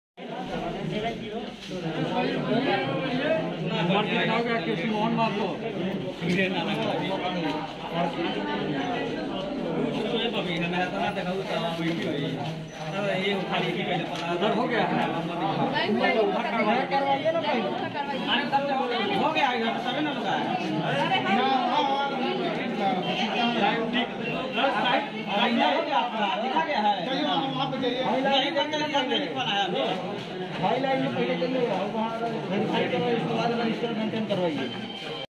bihar me Vaccine lene ke# लिए लोगो का लगा भीड़ लकडॉन गाइडलाइन का धज्जिया उड़ाते हुवे kisan kheti
bihar me Vaccine lene ke# लिए लोगो का लगा भीड़ लकडॉन गाइडलाइन का धज्जिया उड़ाते हुवे kisan kheti baadi